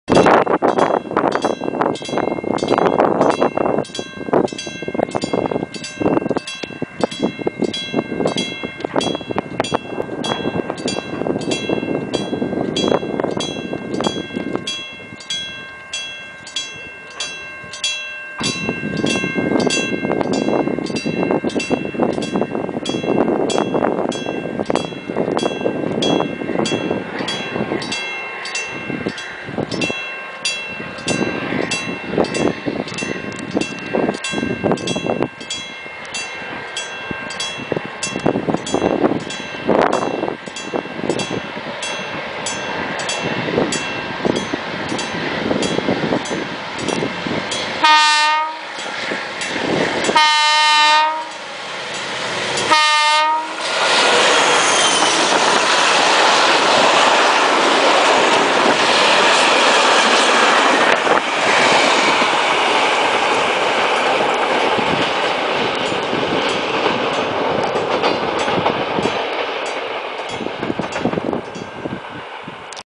Α469 at Amfiklia
MLW A469 passes from Amfiklia station with  Military Vehicles